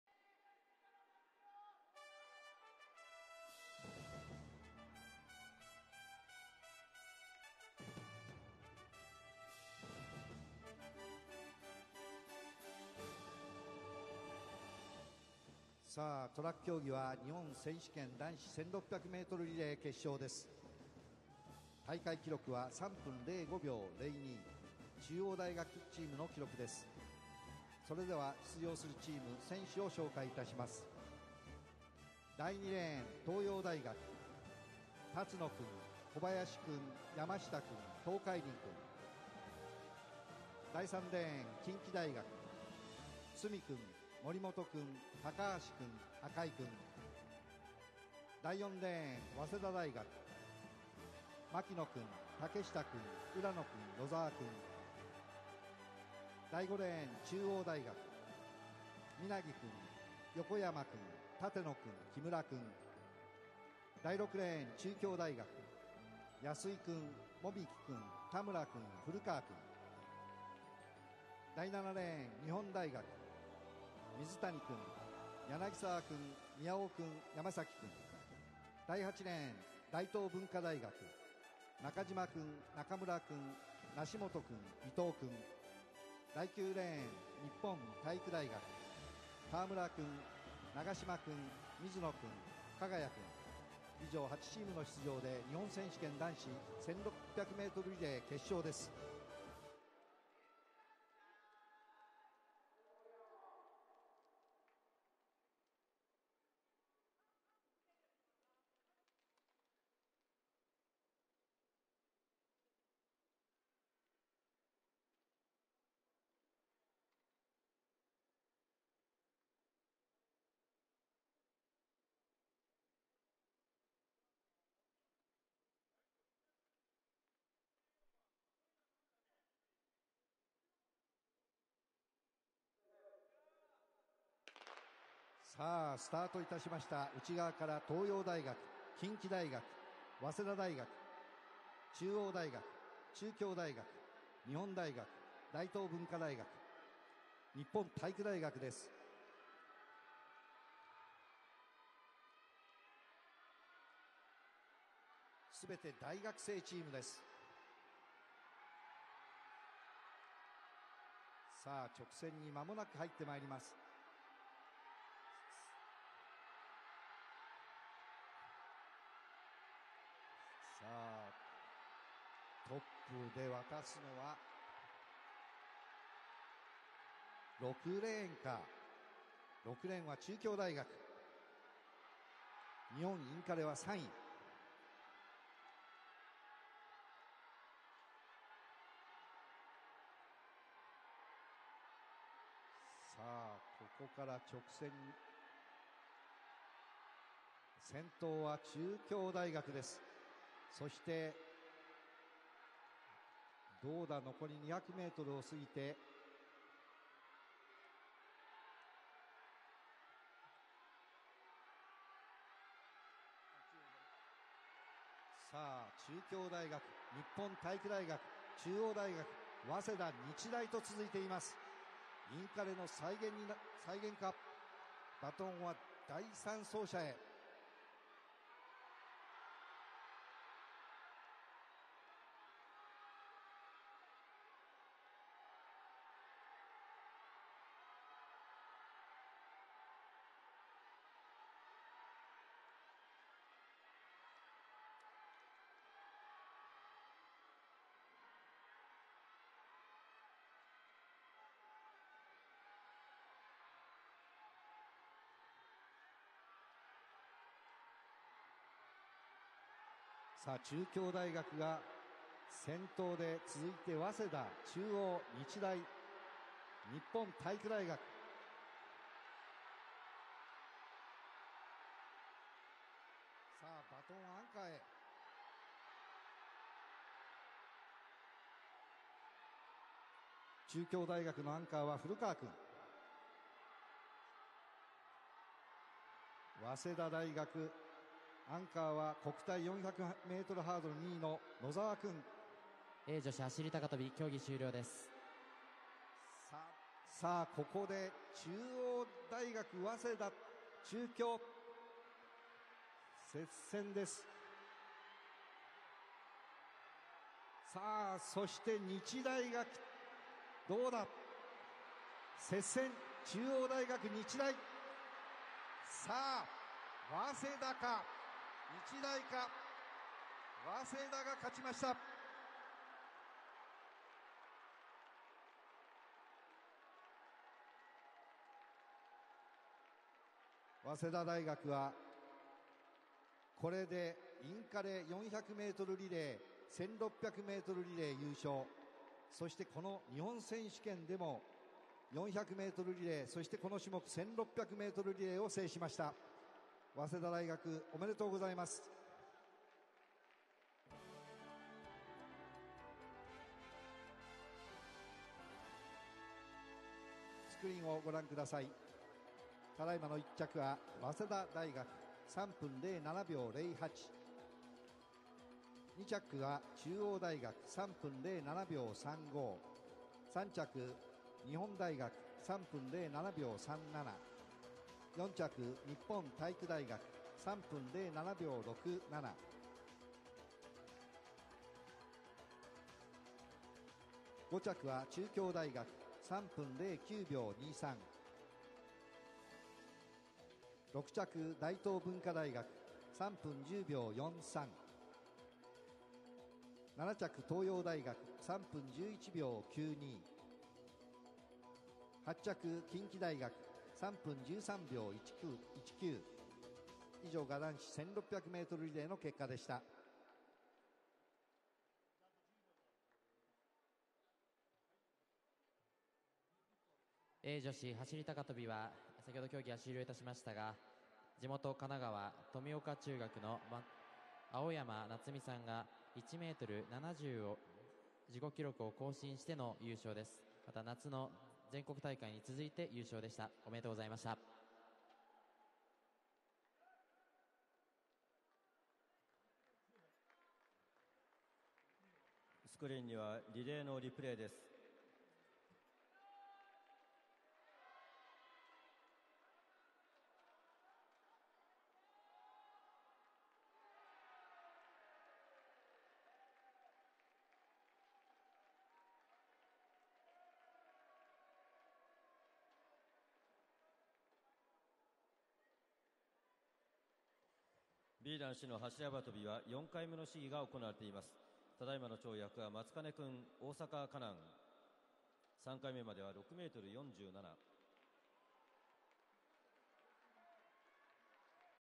第96回日本選手権リレー 男子 4x400mリレー決勝
順位 ﾚｰﾝ チーム名
ﾅﾝﾊﾞｰ 競 技 者
所属団体名 記録 備考
1 4 早大
86 牧野 武 愛 知 3:07.08
81 竹下 裕希 福 岡
85 浦野 晃弘 広 島
87 野澤 啓佑 山 梨

2 5 中央大
117 皆木 佑太 神奈川 3:07.35
118 横山 直広 静 岡
113 舘野 哲也 茨 城
111 木村 淳 沖 縄

3 7 日大
203 水谷 祐己 東 京 3:07.37
192 柳澤 純希 北海道
196 宮尾 幸太郎 東 京
199 山﨑 謙吾 埼 玉

4 9 日体大
217 川村 将平 静 岡 3:07.67
218 長島 正憲 神奈川
220 水野 龍彦 神奈川
216 加賀谷 秀明 千 葉

5 6 中京大
58 安井 一樹 愛 知 3:09.23
60 籾木 勝吾 宮 崎
55 田村 朋也 愛 知
59 古川 拓夢 岐 阜

6 8 大東大
264 中島 雅達 埼 玉 3:10.43
261 中村 祐太 神奈川
262 梨本 真輝 千 葉
258 伊藤 卓 静 岡

7 2 東洋大
13 辰野 充 千 葉 3:11.92
12 小林 将一 埼 玉
9 山下 永吉 静 岡
8 東海林 拓也 静 岡

8 3 近畿大
208 角 達也 大 阪 3:13.19
210 森本 大智 徳 島
214 高橋 央 愛 媛
211 赤井 勝哉 兵 庫

主催 公益財団法人日本陸上競技連盟
主管 神奈川陸上競技協会
協賛 株式会社アシックス 株式会社ナイキジャパン 大塚製薬株式会社 日本航空 株式会社ニシ・スポーツ 株式会社セレスポ
期日 2012年10月26日(金)～10月28日(日)
場所 日産スタジアム